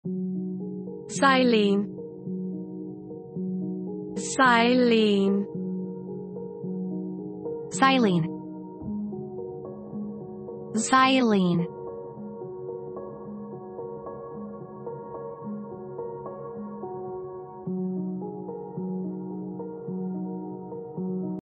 XYLENE pronunciation • How to pronounce XYLENE
How to say Xylene in British English and American English?